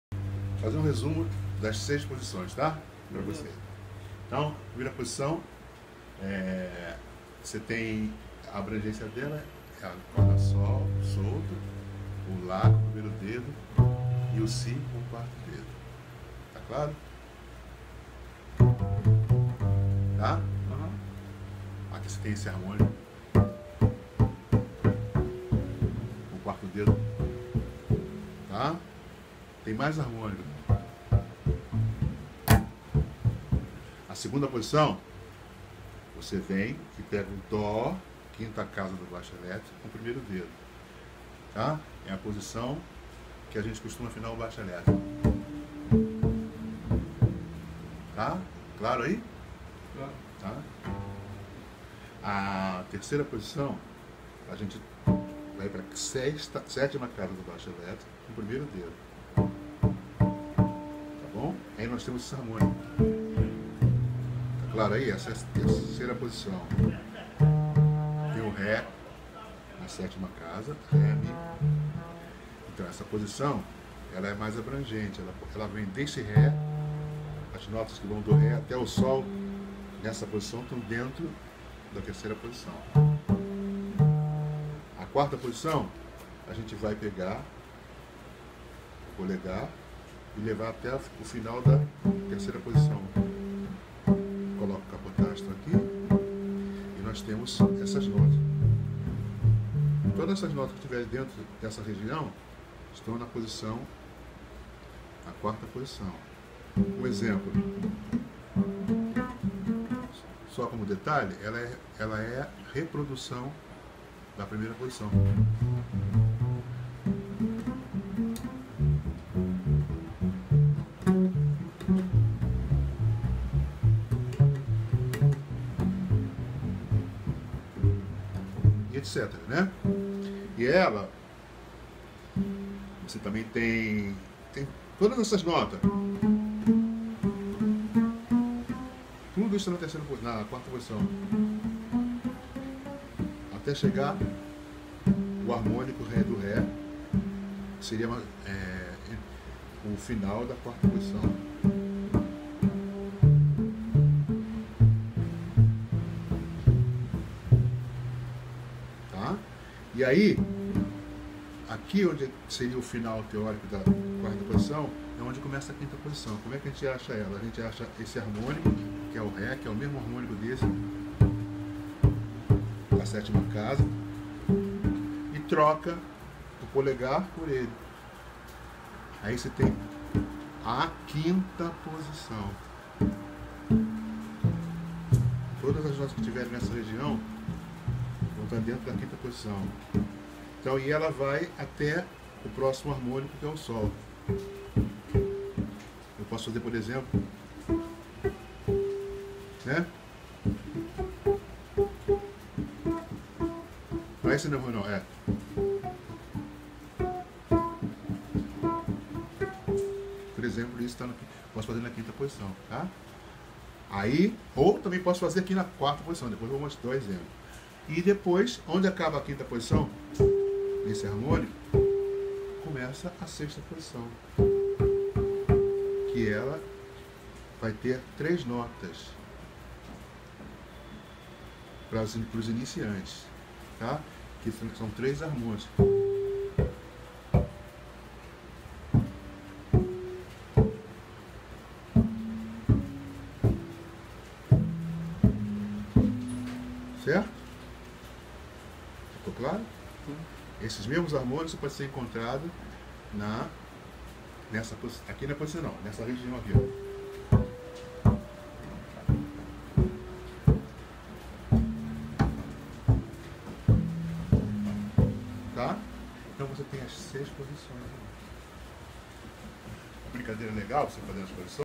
Baixo acustico (Posições)
Uma bela tarde estou eu em casa e recebo a visita deste mestre da música , Jorge Oscar baixista acustico , generosamente ele deixa essas dicas preciosas ... valeu Jorge !!